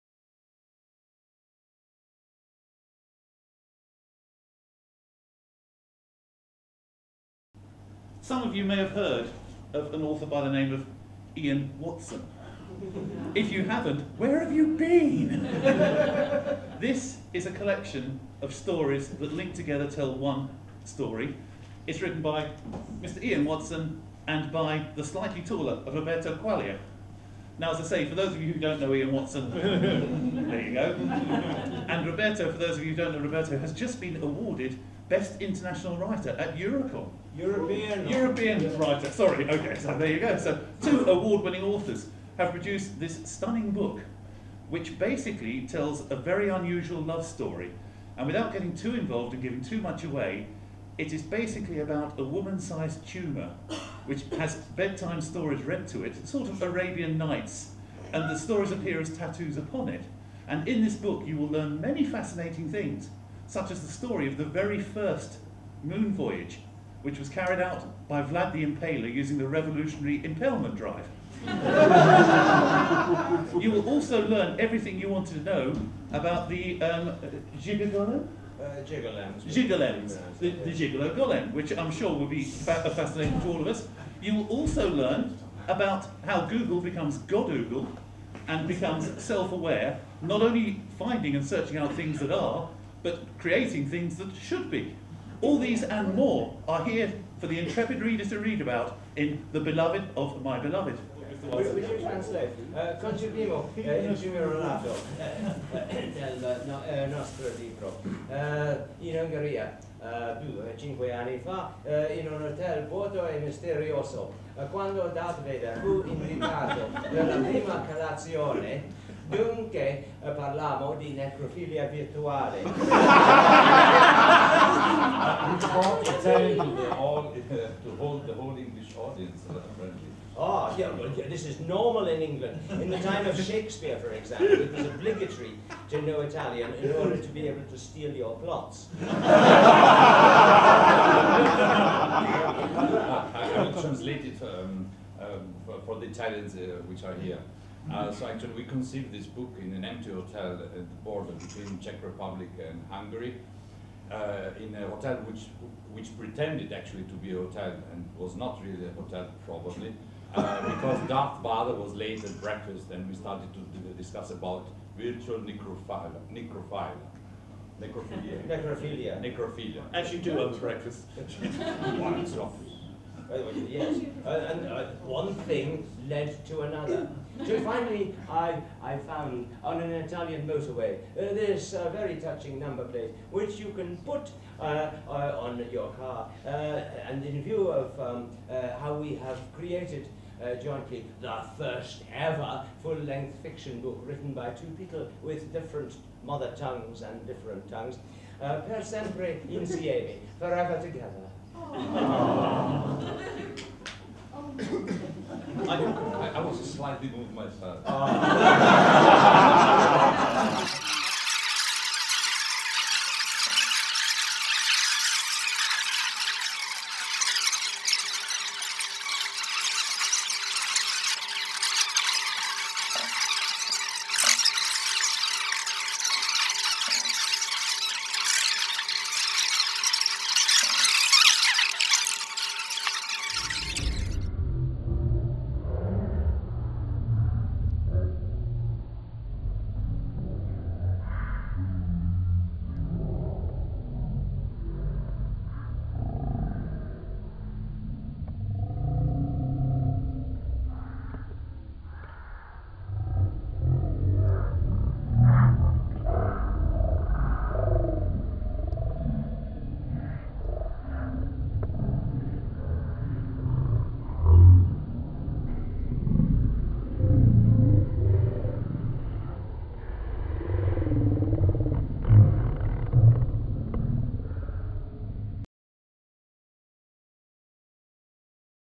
UK book lauch of "The beloved of my beloved" by Ian Watson & Roberto Quaglia
April 11, 2009 - Ian Whates, of Newcon Press, launches Ian Watson & Roberto Quaglia's surrealistic erotic and anti-erotic SF book "The Beloved of my Beloved", during Eastercon 2009 in Bradford, UK